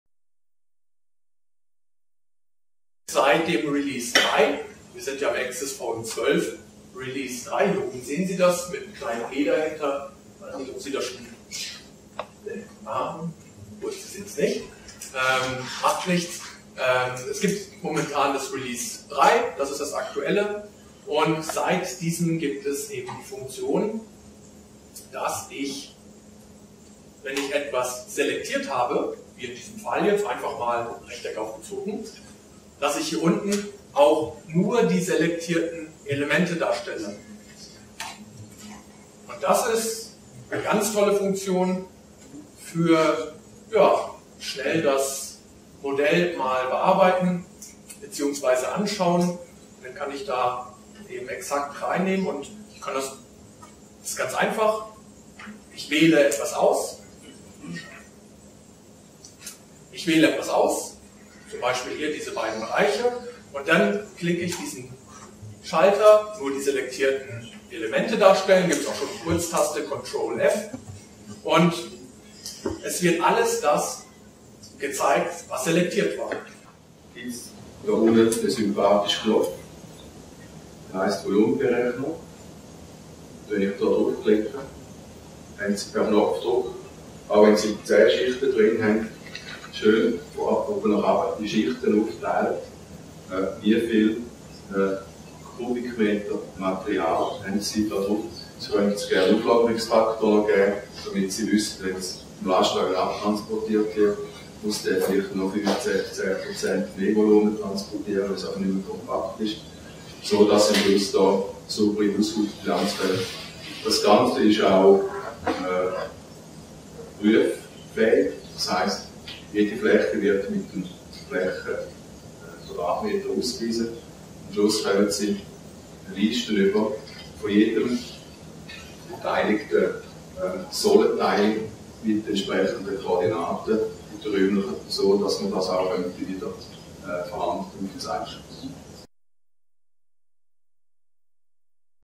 Anwendertreffen 2015 Basel [de]
Ausschnitt aus den Vorträgen am Anwedertreffen 2015 in Basel von Dirk Mennenga und Peter Walker zu AxisVM und DC-Integra